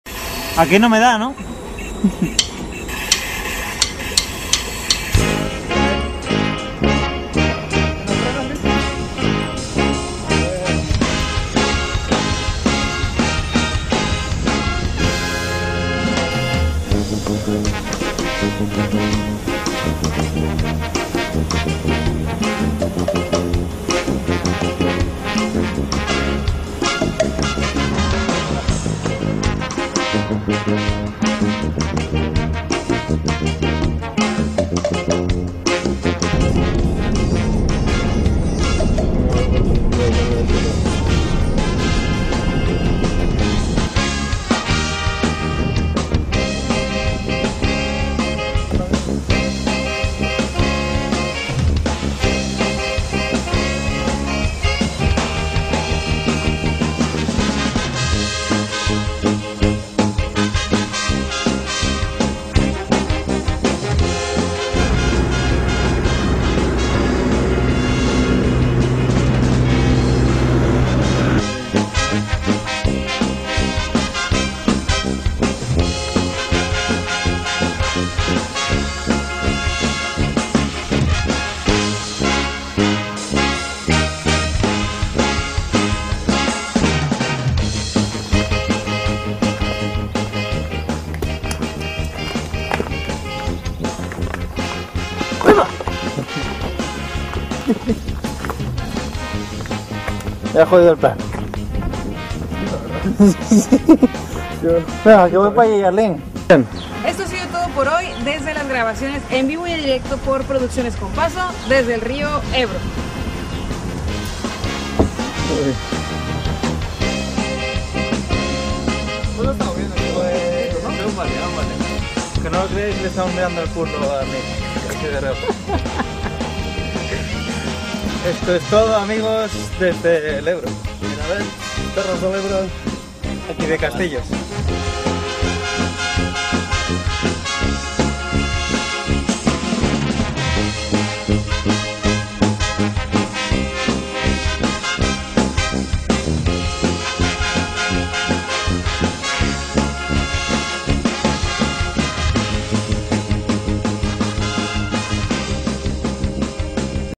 Kompaso de ruta por el Ebro
Producciones Kompaso de ruta por el Ebro de la mano de Jesús Moncada